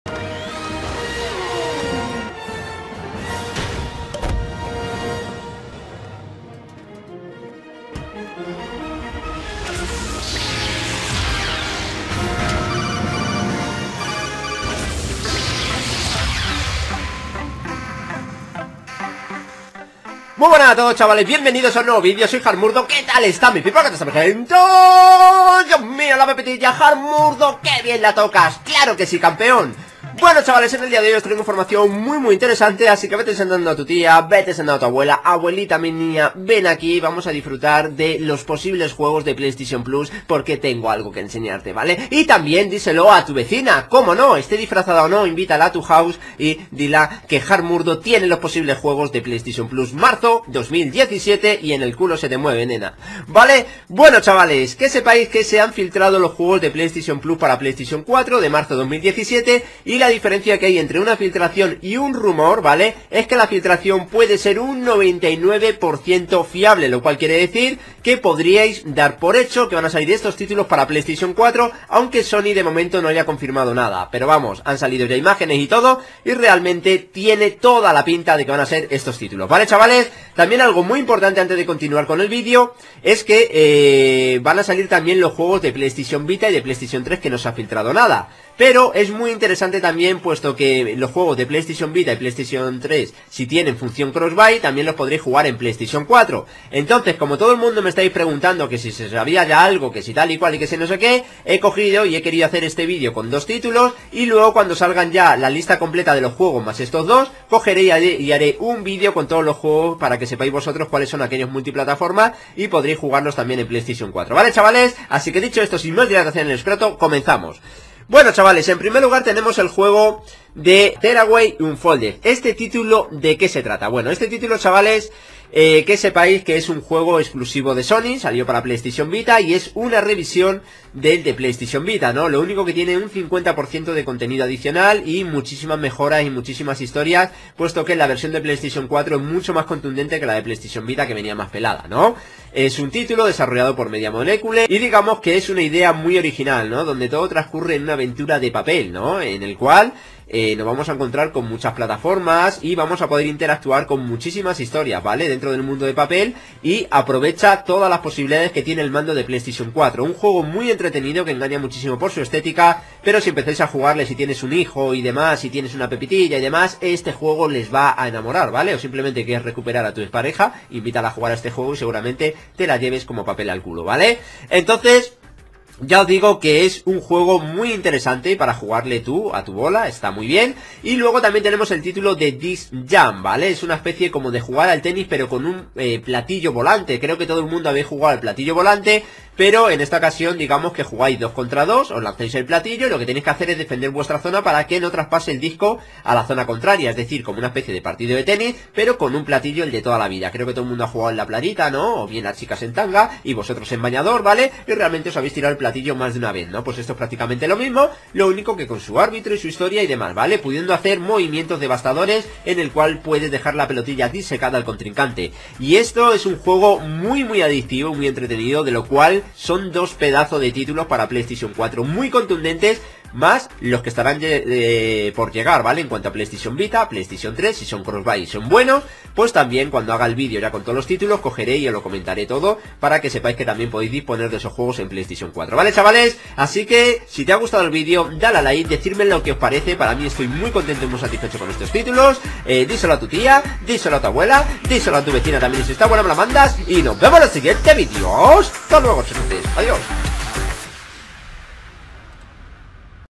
¡¡¡JUEGOS PS PLUS GRATIS MARZO 2017!!! Hardmurdog - Filtración - Ps4
✔ Tiendas europeas podrían estar revelando parte de los juegos de PlayStation Plus que llegarían el próximo mes de marzo. Este anuncio revelaría así que los usuarios del servicio recibirían en PlayStation 4, Tearaway Unfolded y Disc Jam. El resto de juegos todavía no están confirmados, pero todo indica que el anuncio oficial se produciría en los próximos días.
✔ Si te ha gustado el vídeo díselo a tu tía, a tu abuela y a tu vecina...